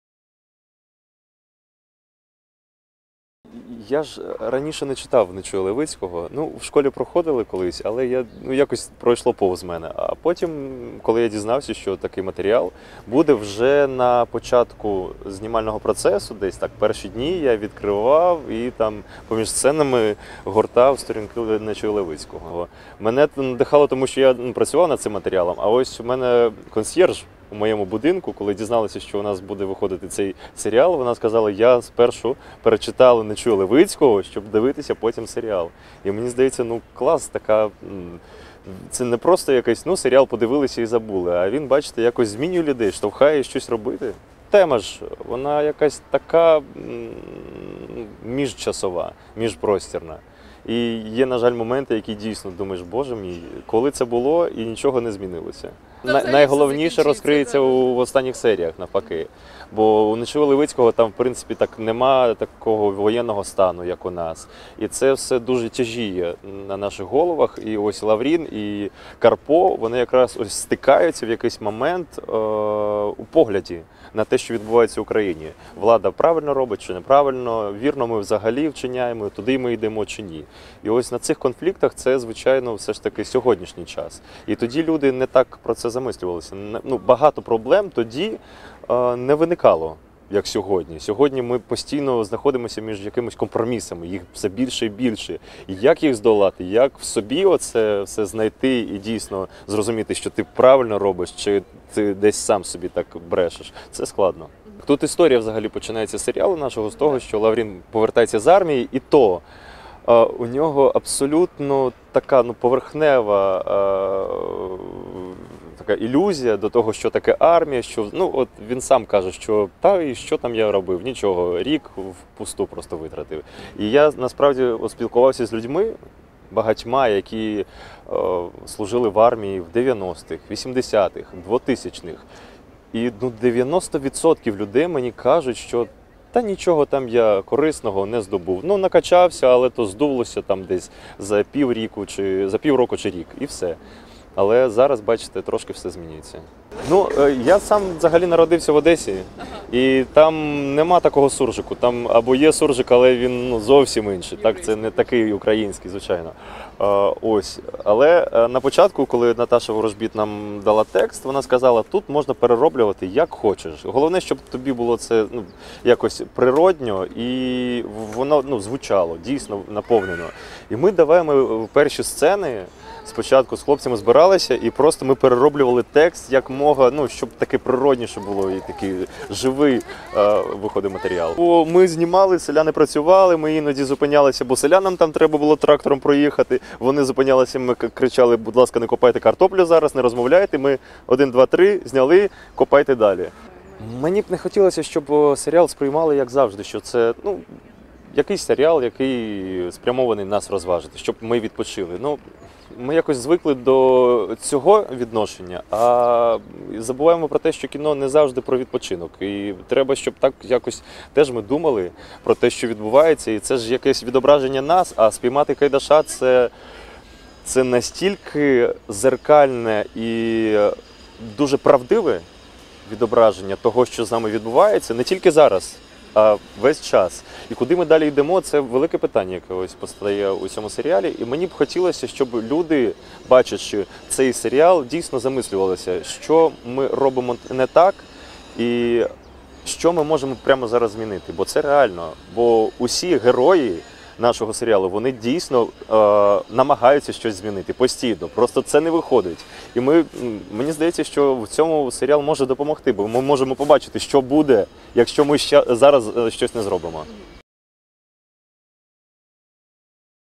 Интервью с актером сериала Поймать Кайдаша Григорием Баклановым
Актер сериала Поймать Кайдаша Григорий Бакланов в интервью Фактам ICTV рассказала о работе над ролью Лаврина,  адаптации повести Нечуй-Левицкого Кайдашева семья и ожидании от реакции зрителей.